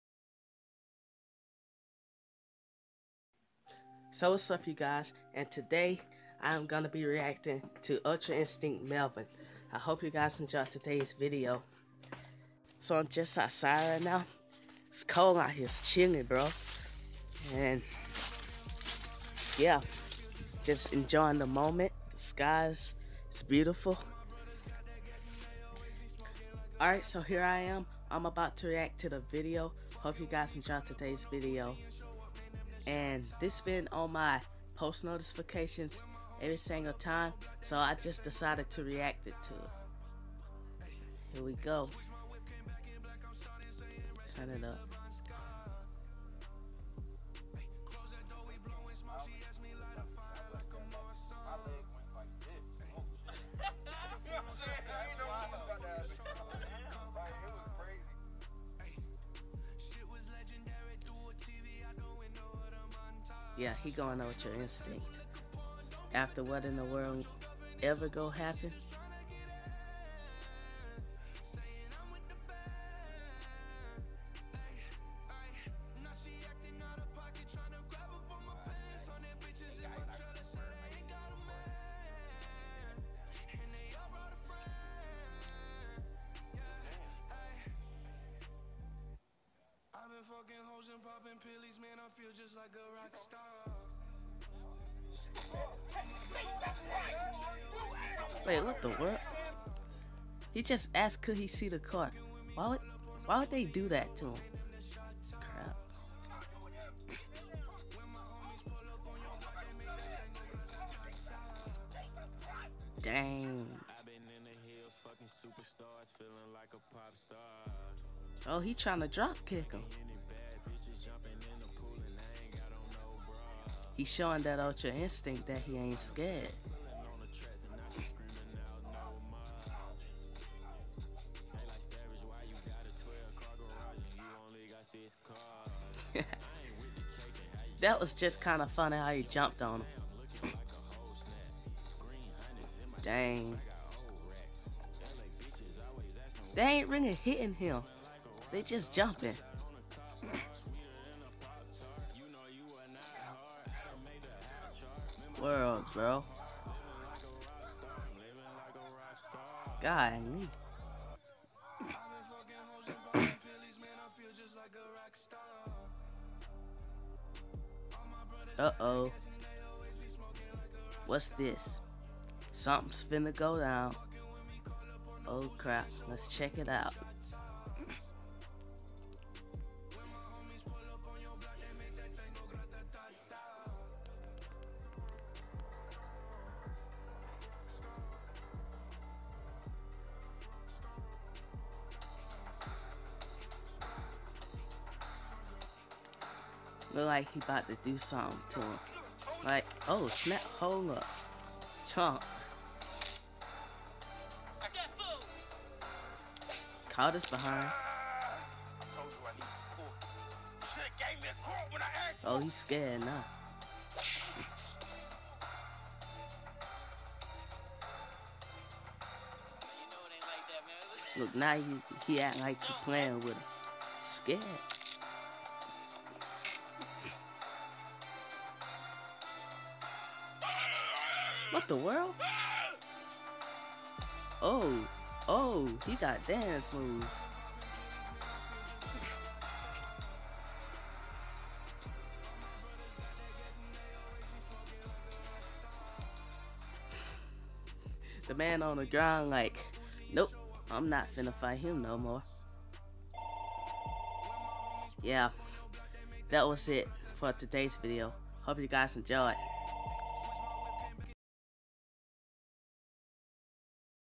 Ultra Instinct Melvin reaction
So this video was always on my YouTube screen and I decided to react to it I enjoyed the video from the guys who created this parody it was funny and came from dragon ball super and I am a fan of anime and comics so please like and subscribe this is for anyone watching